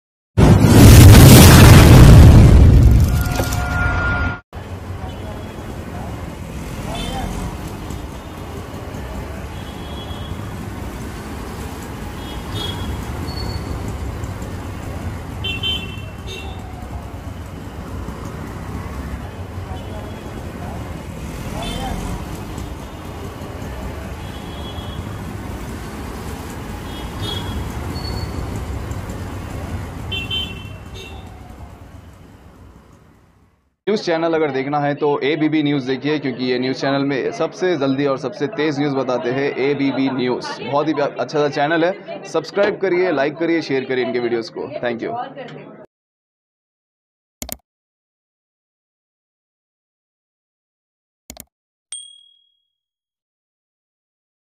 Khaleel Ahmed Has Been Arrested By Chandryangutta Police For Circulating Fake Photo Of Barrister
Malakpet Akberbagh Advocate Khaleel Ahmed Has Been Arrested By Chandryangutta Police For Circulating Fake Photo Of Barrister AsaduddinOwaisiSahab..